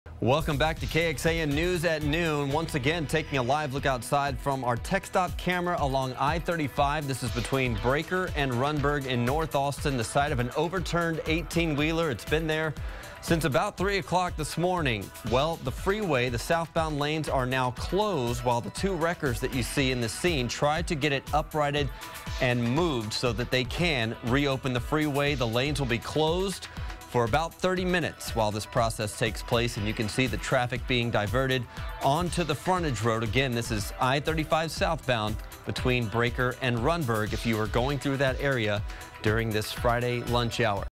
Rig blocking I-35 towed from highway
A big rig flipped on southbound Interstate 35, between Braker and Rundberg early Friday morning -- snarling traffic. A tow truck finally managed to haul the wrecked truck from freeway just before 1 p.m.